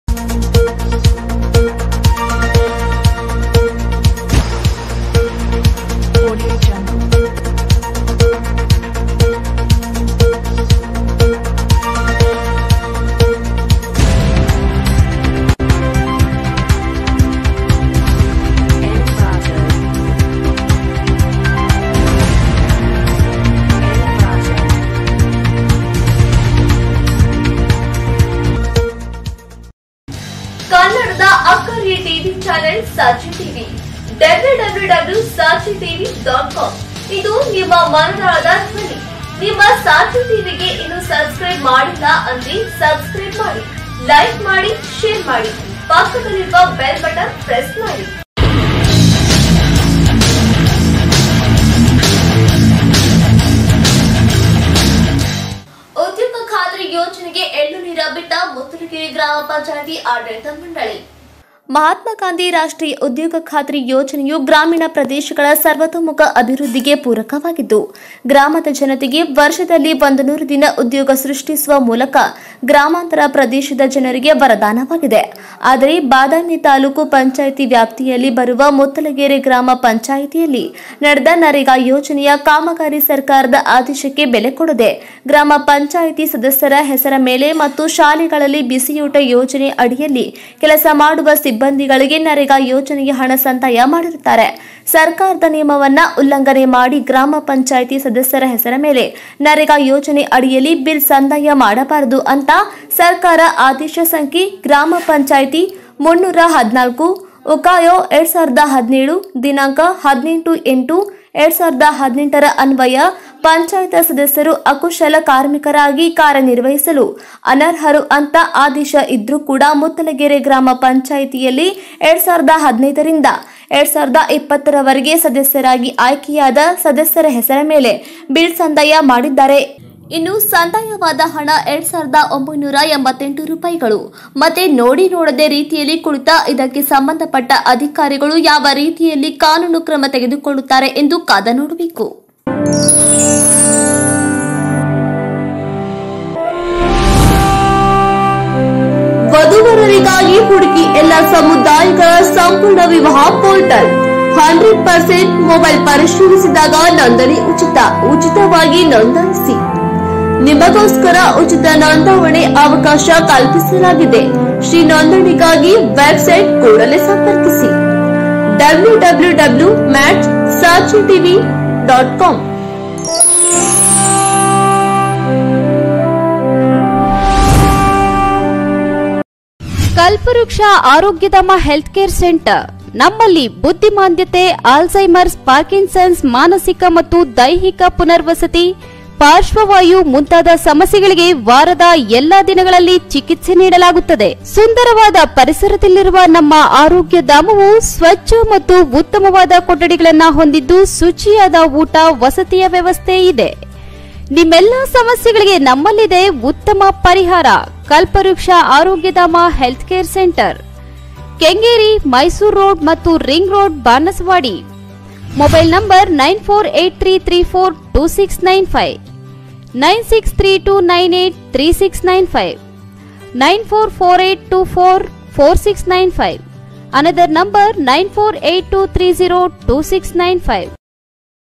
ಉದ್ಯೋಗ ಖಾತ್ರಿ ಯೋಜನೆಗೆ ಎಳ್ಳು ನೀರು ಬಿಟ್ಟ ಮುತ್ತಲಗೇರಿ ಗ್ರಾಮ ಪಂಚಾಯತ್ ಆಡಳಿತ ಮಂ, sachi tv
ಉದ್ಯೋಗ ಖಾತ್ರಿ ಯೋಜನೆಗೆ ಎಳ್ಳು ನೀರು ಬಿಟ್ಟ  ಮುತ್ತಲಗೇರಿ ಗ್ರಾಮ ಪಂಚಾಯತ್ ಆಡಳಿತ ಮಂಡಳಿ.

ಬಾದಾಮಿ :-  ಮಹಾತ್ಮಾ ಗಾಂಧಿ ರಾಷ್ಟ್ರೀಯ ಉದ್ಯೋಗ ಖಾತ್ರಿ ಯೋಜನೆಯು ಗ್ರಾಮೀಣ ಪ್ರದೇಶಗಳ ಸರ್ವತೋಮುಖ ಅಭಿವೃದ್ಧಿಗೆ ಪೂರಕವಾಗಿದ್ದು, ಗ್ರಾಮದ ಜನತೆಗೆ ವರ್ಷದಲ್ಲಿ 100 ದಿನ ಉದ್ಯೋಗ ಸೃಷ್ಟಿಸುವ ಮೂಲಕ ಗ್ರಾಮಾಂತರ ಪ್ರದೇಶದ ಜನರಿಗೆ ವರದಾನವಾಗಿದೆ ಆದರೆ ಬಾದಾಮಿ ತಾಲೂಕ ಪಂಚಾಯತಿ ವ್ಯಾಪ್ತಿಯಲ್ಲಿ ಬರುವ ಮುತ್ತಲಗೇರಿ ಗ್ರಾಮ ಪಂಚಾಯತಿಯಲ್ಲಿ ನಡೆದ ನರೇಗಾ ಯೋಜನೆಯ‌ ಕಾಮಗಾರಿಯ ಸರ್ಕಾರದ ಆದೇಶ ಕ್ಕೆ ಬೆಲೆ ಕೊಡದೆ ಗ್ರಾಮ ಪಂಚಾಯತಿ ಸದಸ್ಯರ ಹೆಸರ ಮೇಲೆ ಮತ್ತು ಶಾಲೆಗಳಲ್ಲಿ ಬಿಸಿಯೂಟ ಯೋಜನೆ ಅಡಿಯಲ್ಲಿ ಕೆಲಸ ಮಾಡುವ ಸಿಬ್ಬಂದಿಗಳಿಗೆ ನರೇಗಾ ಯೋಜನೆಯ ಹಣ ಸಂದಾಯ ಮಾಡಿರುತ್ತಾರೆ. ಸರಕಾರದ ನಿಯಮ ವನ್ನು ಉಲ್ಲಂಘನೆ ಮಾಡಿ ಗ್ರಾಮ ಪಂಚಾಯತಿ ಸದಸ್ಯರ ಹೆಸರ ಮೇಲೆ ನರೇಗಾ ಯೋಜನೆ ಅಡಿಯಲ್ಲಿ ಬಿಲ್ ಸಂದಾಯ ಮಾಡಬಾರದು ಅಂತ ಸರ್ಕಾರ ಆದೇಶ ಸಂಖ್ಯೆ ಗ್ರಾ ಅ ಪ 314/ಉಖಾಯೋ 2017 ದಿನಾಂಕ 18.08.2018 ರನ್ವಯ ಪಂಚಾಯತ ಸದಸ್ಯರು ಅಕುಶಲ ಕಾರ್ಮಿಕರಾಗಿ ಕಾರ್ಯನಿರ್ವಹಿಸಲು ಅನರ್ಹರರು ಅಂತ ಆದೇಶ ಇದ್ರು  ಕೂಡಾ ಮುತ್ತಲಗೇರಿ ಗ್ರಾಮ ಪಂಚಾಯತಿ ಯಲ್ಲಿ 2015 ರಿಂದ 2020 ರ ವರೆಗೆ ಸದಸ್ಯರಾಗಿ ಆಯ್ಕೆಯಾದ ಸದಸ್ಯರ ಹೆಸರ ಮೇಲೆ ಬಿಲ್ ಸಂದಾಯ ಮಾಡಿದ್ದಾರೆ. ಸದಸ್ಯರಾದ

01) ಯಲ್ಲವ್ವ ಸಿದ್ದಪ್ಪ ಹಣಮಸಾಗರ (ಗ್ರಾಮ ಪಂಚಾಯತಿ ಸದಸ್ಯರು) ಇವರ ಜಾಬ್ ಕಾರ್ಡ್ ನಂಬರ್ KN-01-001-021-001/12809.ಮತ್ತೆ NMR ಗಳು 5075, 8689, 10172, 3352, 3725, 9078, 9162, 1708 ಒಟ್ಟು ಇವರ ಹೆಸರಲ್ಲಿ ಸಂದಾಯವಾದ   ಒಟ್ಟು ಮೊತ್ತ 20625 ರೂಪಾಯಿಗಳು.

 02) ಯಲ್ಲವ್ವ  ಮಲ್ಲಪ್ಪ ಮೇಲನಾಡ (ಗ್ರಾಮ ಪಂಚಾಯತಿ ಸದಸ್ಯರು)ಇವರ ಜಾಬ್ ಕಾರ್ಡ್ ನಂಬರ್ KN-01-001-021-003/1111. NMR ಗಳು 791, 1413, 6503, 4836, 6471, 8347 ಇವರ ಹೆಸರಲ್ಲಿ  ಒಟ್ಟು ಸಂದಾಯವಾದ  ಹಣ 8715 ರೂಪಾಯಿಗಳು

03) ರಂಗವ್ವ ಲಕ್ಷ್ಮಣ ವಾಲಿಕಾರ  (ಗ್ರಾಮ ಪಂಚಾಯತಿ ಸದಸ್ಯರು) ಇವರ ಜಾಬ್ ಕಾರ್ಡ್
KN-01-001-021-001/12384 NMR ಗಳು 1154, 1160, 8015, 1950, 3724, 1546, 2798 ಇವರ ಹೆಸರಲ್ಲಿ ಒಟ್ಟು ಸಂದಾಯವಾದ ಹಣ 15749ರೂಪಾಯಿಗಳು

04) ಬಸವ್ವ  ತಾಯಪ್ಪ ಮಾದರ ಇವರು ಸರ್ಕಾರಿ ಹಿರಿಯ ಪ್ರಾಥಮಿಕ ಶಾಲೆ ಮುತ್ತಲಗೇರಿ ಅಡುಗೆ ಸಹಾಯಕಿಯಾಗಿ ಕೆಲಸಮಾಡುತ್ತಾರೆ. ಇವರ ಜಾಬ್ ಕಾರ್ಡ್ ನಂಬರ್ KN-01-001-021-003/770 NMR ಗಳು 1602, 8347 ಇವರ ಹೆಸರಲ್ಲಿ ಒಟ್ಟು ಸಂದಾಯವಾದ ಹಣ 2988 ರೂಪಾಯಿಗಳು.ಮತ್ತೆ ನೋಡಿ ನೋಡದೆ ರೀತಿಯಲ್ಲಿ ಕುಳಿತ ಇದಕ್ಕೆ ಸಂಬಂಧ ಪಟ್ಟ ಅಧಿಕಾರಿಗಳು ಯಾವ ರೀತಿಯಲ್ಲಿ ಕಾನೂನು ಕ್ರಮ ತಗೆದುಕೊಳ್ಳುತ್ತಾರೆ ಎಂದು ಕಾದು ನೋಡಬೇಕು.

ವರದಿ :- ಚಿನ್ನಪ್ಪ ಕುಂದರಗಿ